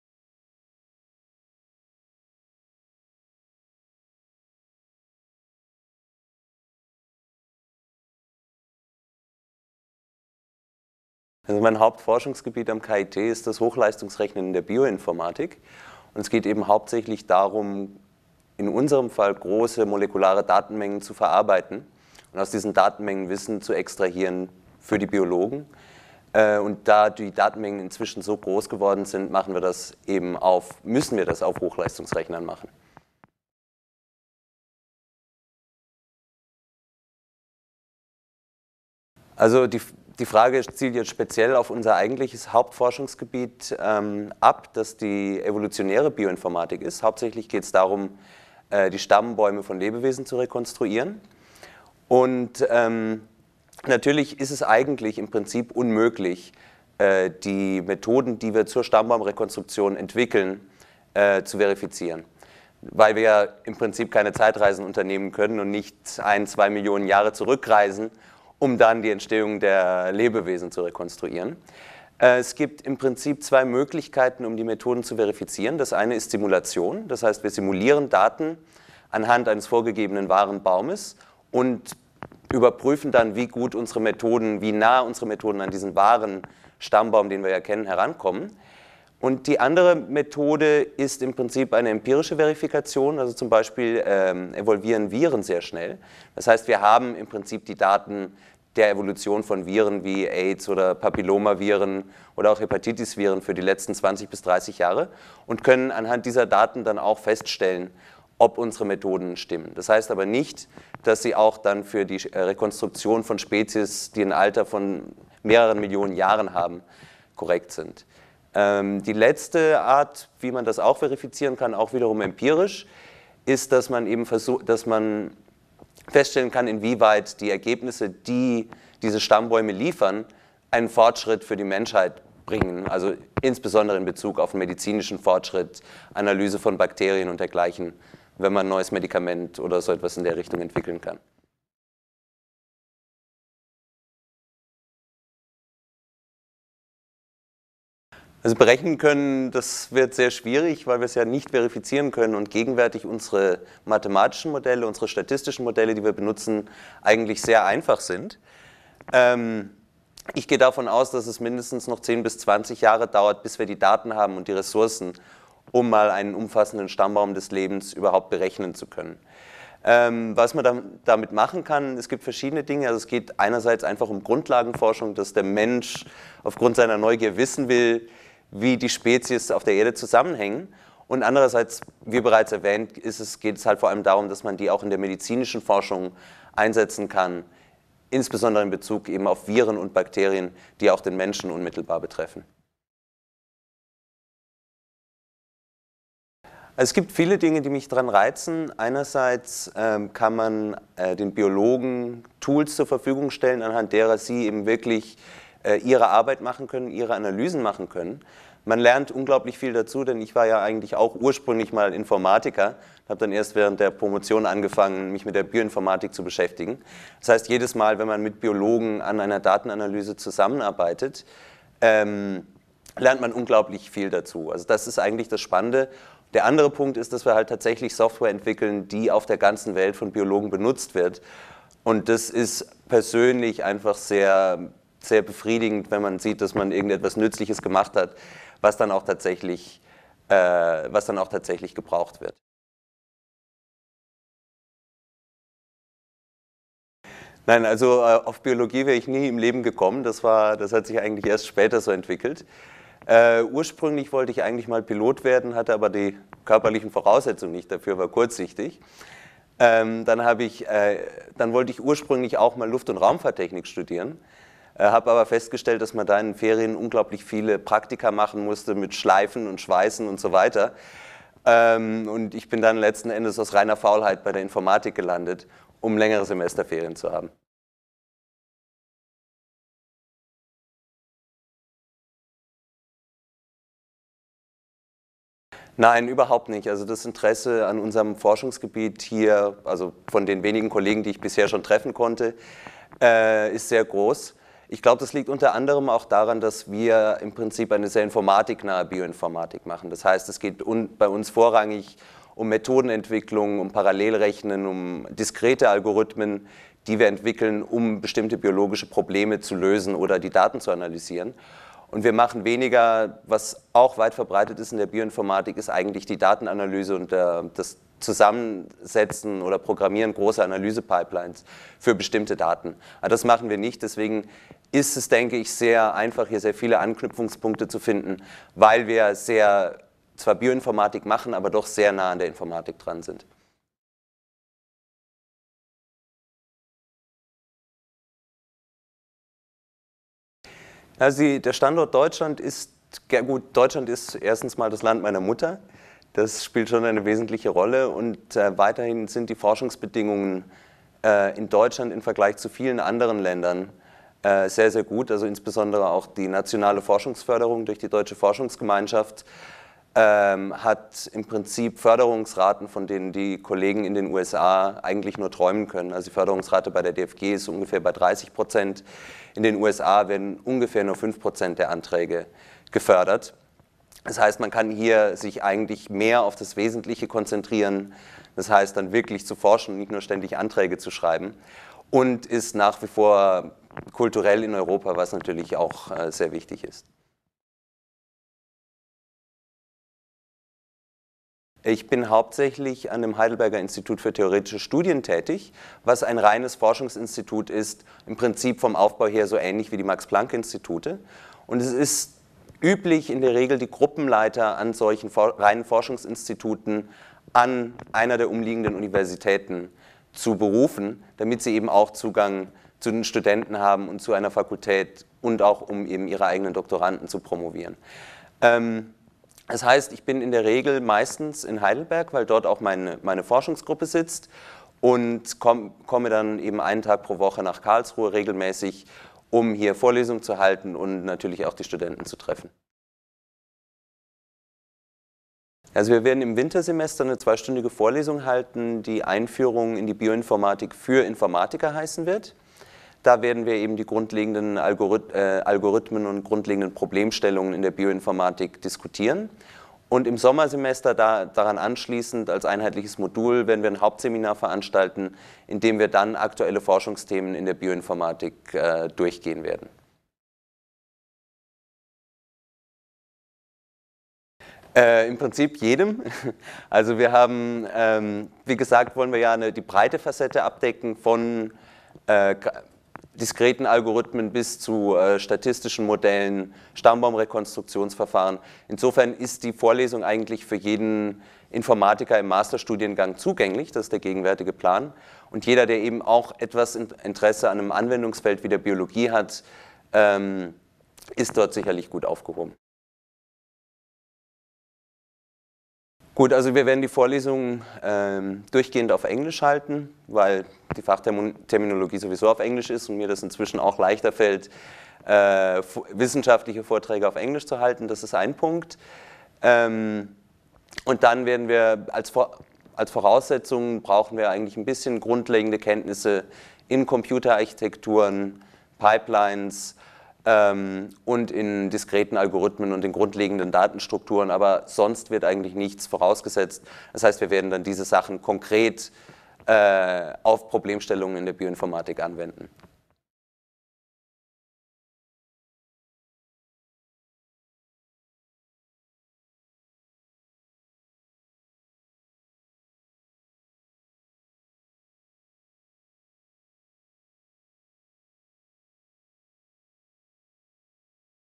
Hochleistungsrechnen in der Bioinformatik
Interview mit Prof. Dr. Alexandros Stamatakis | Karlsruher Institut für Technologie (KIT), Fakultät für Informatik